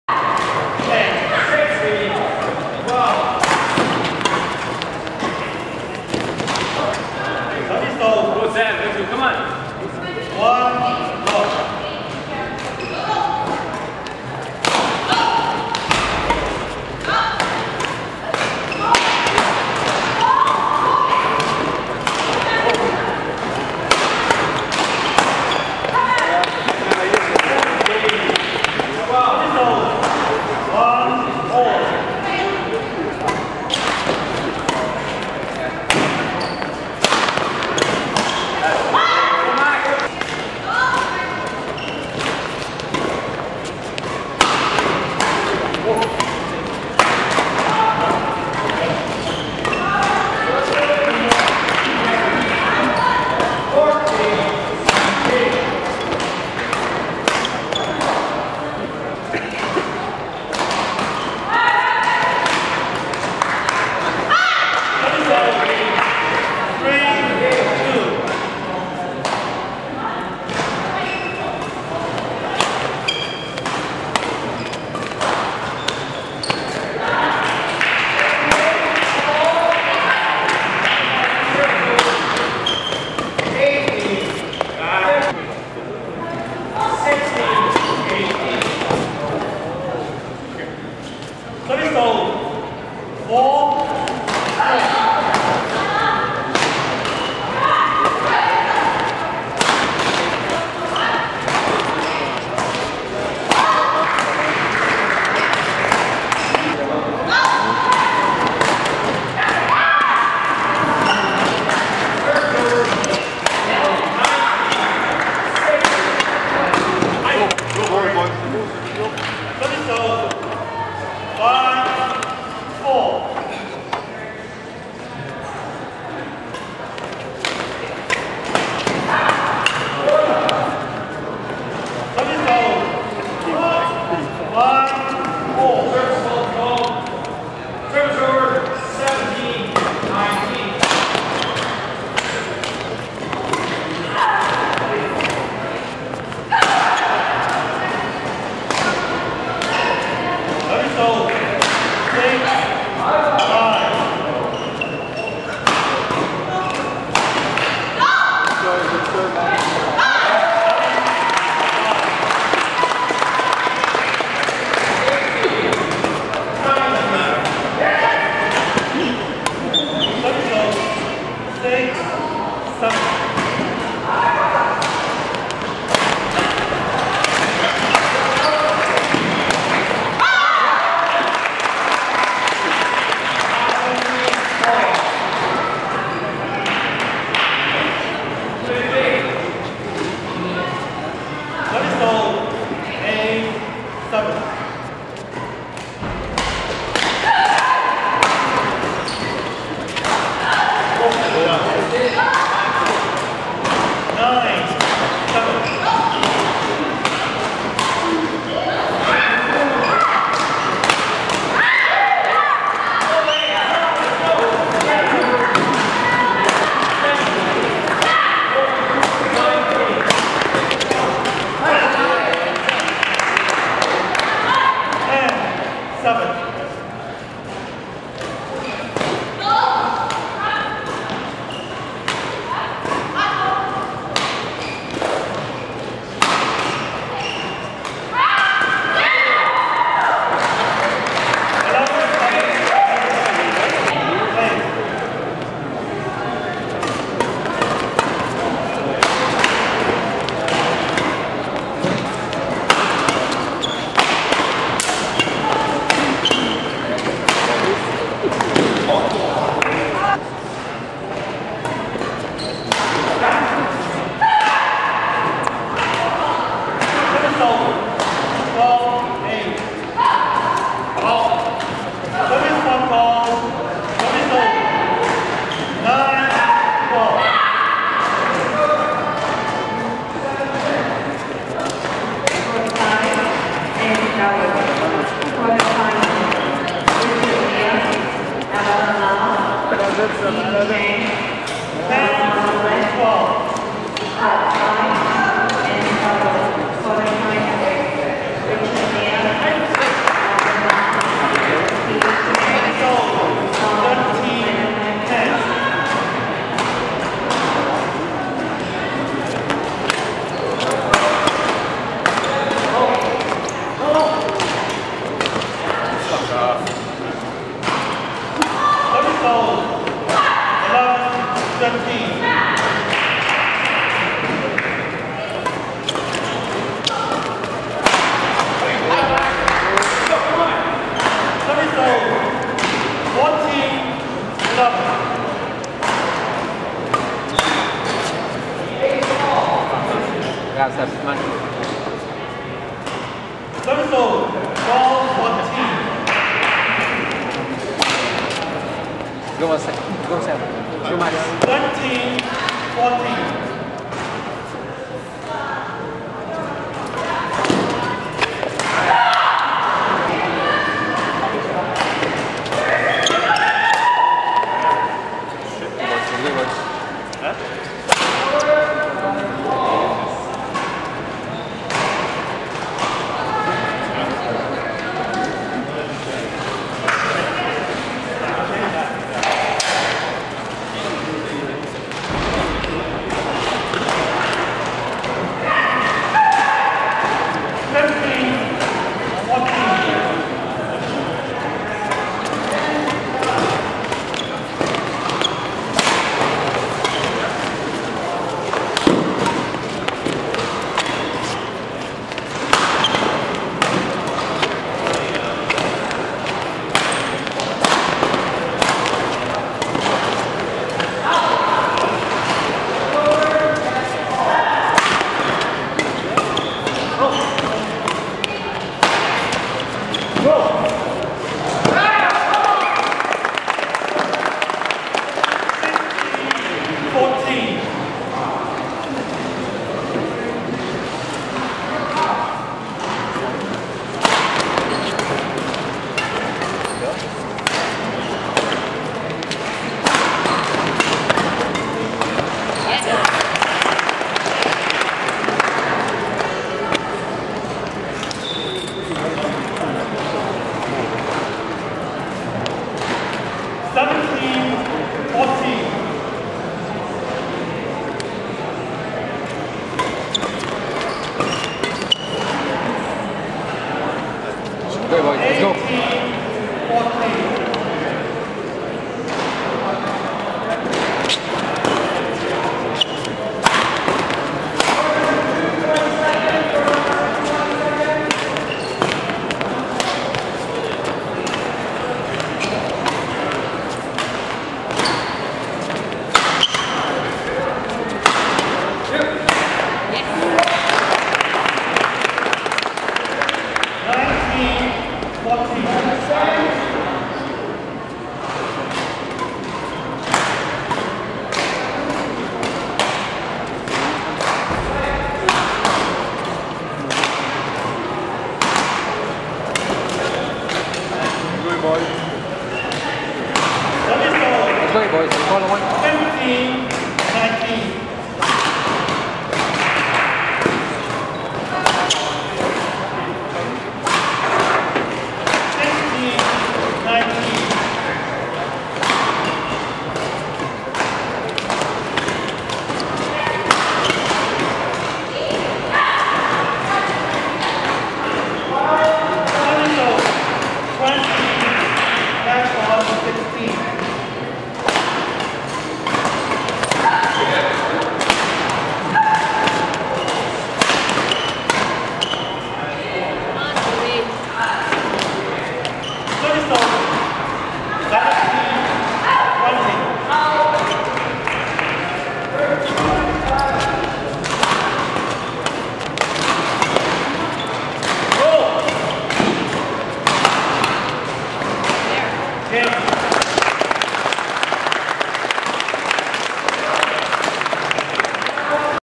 MD QF NgHoloboff v. BelangerRCoutu ('08 Cdn Nationals) Set 2
MD QF Toby Ng/Kyle Holoboff (in black & blue) vs. Maxime Belanger/Sebastien Roshon-Coutu (in white) at the 2008 Canadian Badminton National Championships